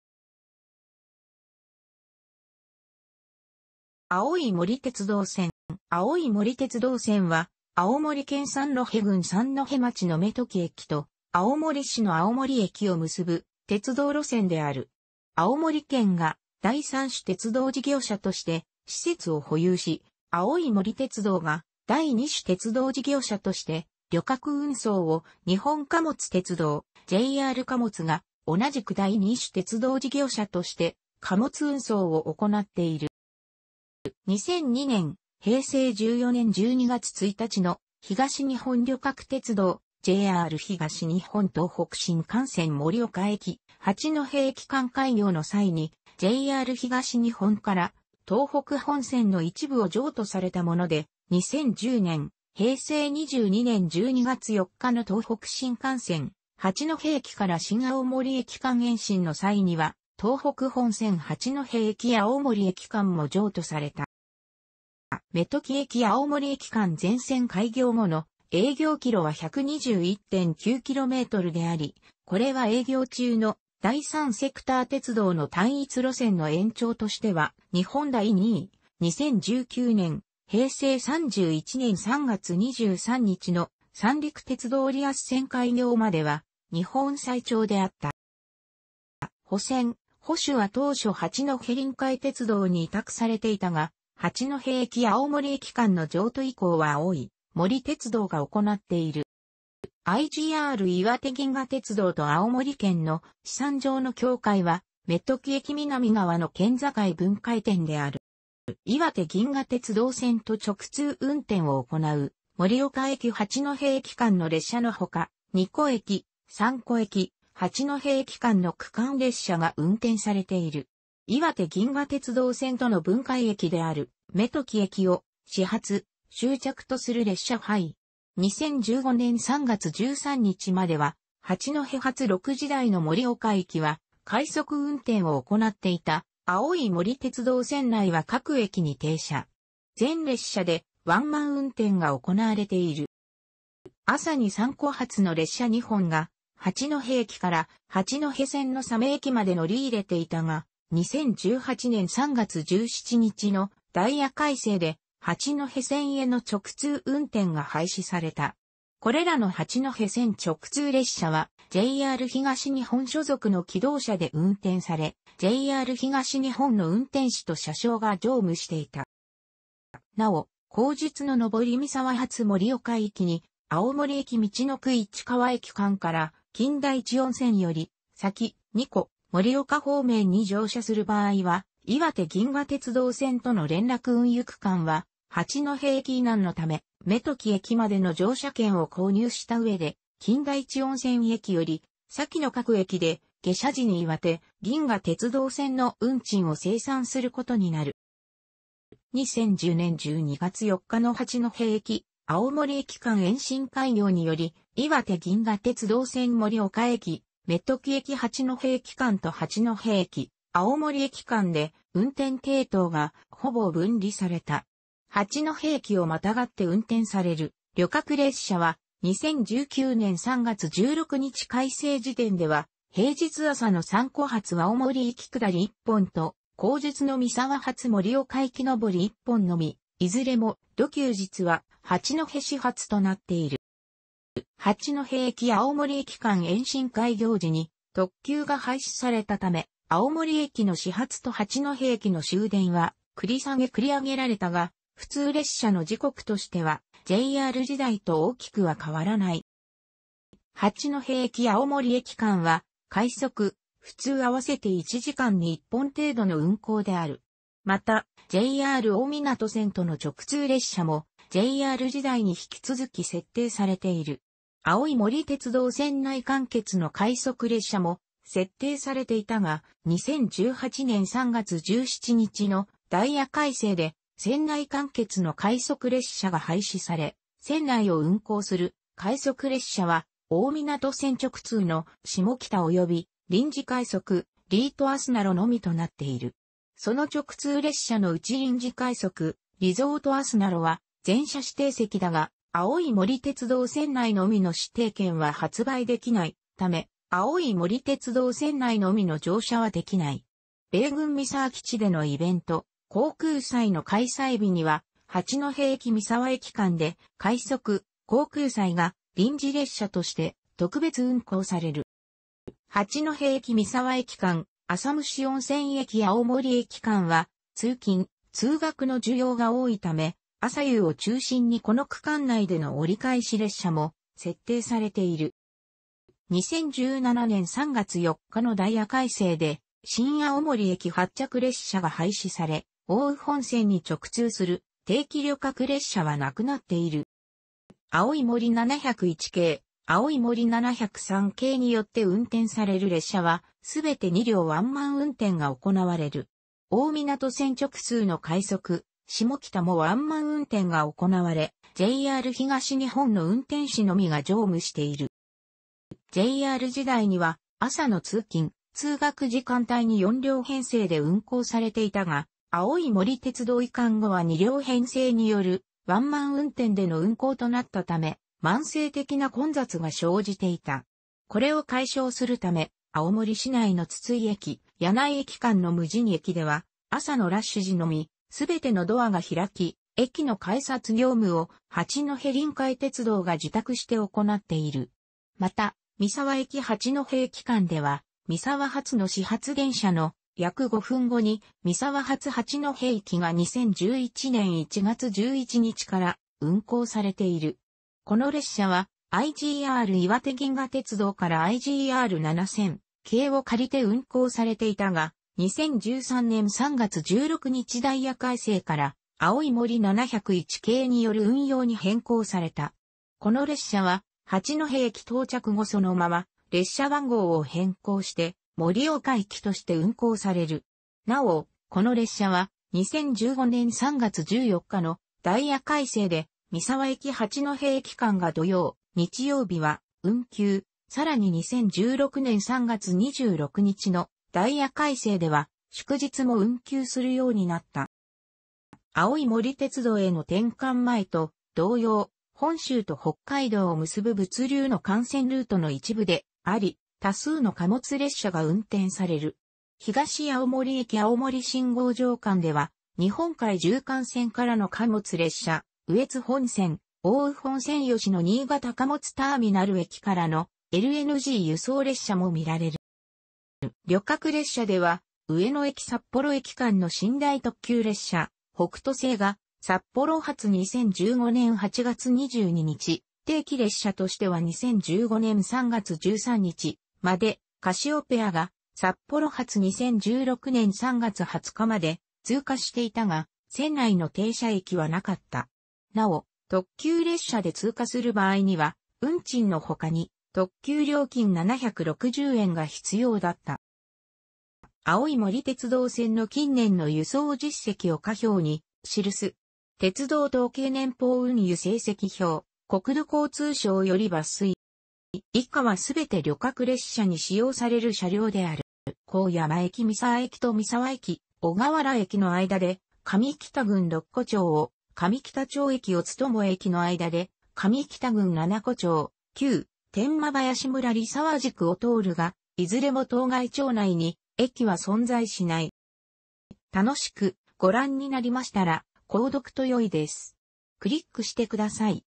青い森鉄道線
青い森鉄道線（あおいもりてつどうせん）は、青森県三戸郡三戸町の目時駅と青森市の青森駅を結ぶ鉄道路線である。青森県が第三種鉄道事業者として施設を保有し、青い森鉄道が第二種鉄道事業者として旅客運送を、日本貨物鉄道（JR貨物）が同じく第二種鉄道事業者として貨物運送を行っている。

2002年（平成14年）12月1日の東日本旅客鉄道（JR東日本）東北新幹線盛岡駅 - 八戸駅間開業の際にJR東日本から東北本線の一部を譲渡されたもので、2010年（平成22年）12月4日の東北新幹線八戸駅 - 新青森駅間延伸の際には、東北本線八戸駅 - 青森駅間も譲渡された。

目時駅 - 青森駅間全線開業後の営業キロは121.9kmであり、これは営業中の第三セクター鉄道の単一路線の延長としては、日本第2位、2019年（平成31年）3月23日の三陸鉄道リアス線 (163.0km) 開業までは日本最長であった。

保線・保守は当初八戸臨海鉄道に委託されていたが、八戸駅 - 青森駅間の譲渡以降は青い森鉄道が行っている。

IGRいわて銀河鉄道と青森県の資産上の境界は目時駅南側の県境分界点である。

いわて銀河鉄道線と直通運転を行う盛岡駅 - 八戸駅間の列車のほか、二戸駅・三戸駅 - 八戸駅間の区間列車が運転されている。いわて銀河鉄道線との分界駅である目時駅を始発・終着とする列車はない。2015年3月13日までは八戸発6時台の盛岡行きは快速運転を行っていた（青い森鉄道線内は各駅に停車）。全列車でワンマン運転が行われている。

朝に三戸発の列車2本が八戸駅から八戸線の鮫駅まで乗り入れていたが、2018年3月17日のダイヤ改正で八戸線への直通運転が廃止された。これらの八戸線直通列車はJR東日本所属の気動車で運転され、JR東日本の運転士と車掌が乗務していた。

なお、後述の上り三沢発盛岡行きに青森駅 - 陸奥市川駅間から金田一温泉より先（二戸・盛岡方面）に乗車する場合は、いわて銀河鉄道線との連絡運輸区間は八戸駅以南のため、目時駅までの乗車券を購入した上で金田一温泉駅より先の各駅で下車時にいわて銀河鉄道線の運賃を精算することになる。

2010年12月4日の八戸駅 - 青森駅間延伸開業により、いわて銀河鉄道線盛岡駅 - 目時駅 - 八戸駅間と八戸駅 - 青森駅間で運転系統がほぼ分離された。八戸駅を跨がって運転される旅客列車は、2019年3月16日改正時点では、平日朝の三戸発青森行き下り1本と後述の三沢発盛岡行き上り1本のみ（いずれも土休日は八戸始発）となっている。

八戸駅 - 青森駅間延伸開業時に、特急が廃止されたため、青森駅の始発と八戸駅の終電は繰り下げ・繰り上げられたが、普通列車の時刻としてはJR時代と大きくは変わらない。

八戸駅 - 青森駅間は快速・普通あわせて1時間に1本程度の運行である。また、JR大湊線との直通列車もJR時代に引き続き設定されている。青い森鉄道線内完結の快速列車も設定されていたが、2018年3月17日のダイヤ改正で、線内完結の快速列車が廃止され、線内を運行する快速列車は大湊線直通の「しもきた」および臨時快速「リゾートあすなろ」のみとなっている。その直通列車のうち臨時快速「リゾートあすなろ」は全車指定席だが、青い森鉄道線内のみの指定券は発売できないため、青い森鉄道線内のみの乗車はできない。米軍三沢基地でのイベント、航空祭の開催日には八戸駅 - 三沢駅間で快速「航空祭」が臨時列車として特別運行される。

八戸駅 - 三沢駅間、浅虫温泉駅 - 青森駅間は、通勤、通学の需要が多いため、朝夕を中心にこの区間内での折り返し列車も設定されている。

2017年3月4日のダイヤ改正で新青森駅発着列車が廃止され、奥羽本線に直通する定期旅客列車はなくなっている。

青い森701系・青い森703系によって運転される列車は、すべて2両ワンマン運転が行われる。大湊線直通の快速「しもきた」もワンマン運転が行われ、JR東日本の運転士のみが乗務している。

JR時代には、朝の通勤・通学時間帯に4両編成で運行されていたが、青い森鉄道移管後は2両編成によるワンマン運転での運行となったため、慢性的な混雑が生じていた。これを解消するため、青森市内の筒井駅 - 野内駅間の無人駅では、朝のラッシュ時のみ、すべてのドアが開き、駅の改札業務を八戸臨海鉄道が受託して行っている。また、三沢駅 - 八戸駅間では...